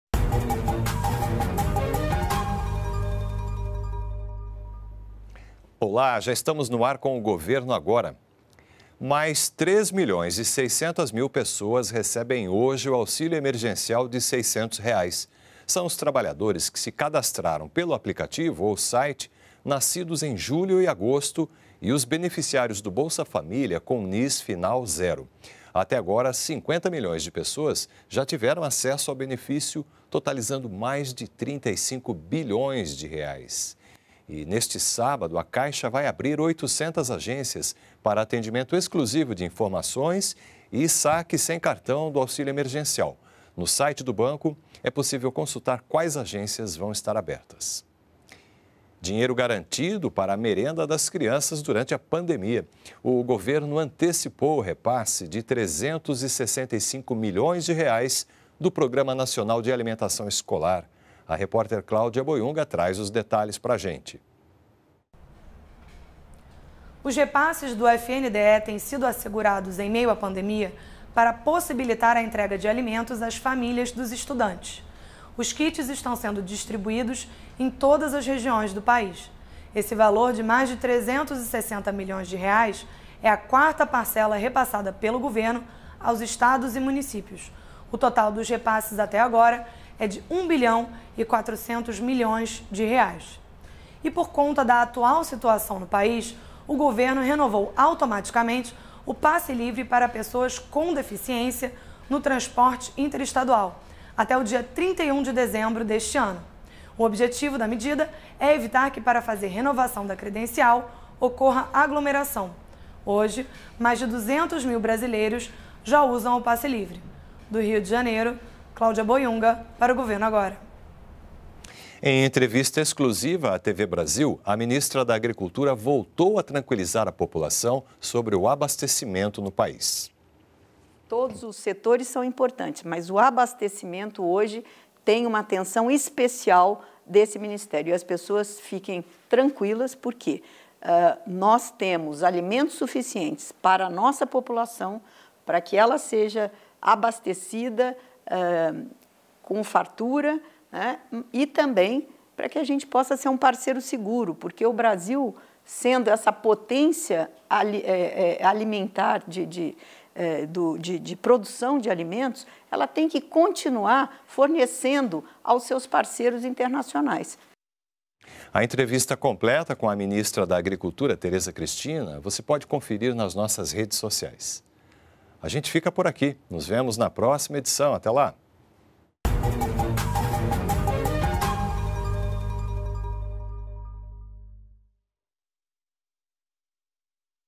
Mais 3,6 milhões pessoas recebem hoje o auxílio emergencial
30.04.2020 -  São os trabalhadores que se cadastraram pelo aplicativo ou site, nascidos em julho e agosto, e os beneficiários do Bolsa Família com NIS final zero; o governo antecipou o repasse de 365 milhões de reais do Programa Nacional de Alimentação Escolar. Veja os destaques desta quinta.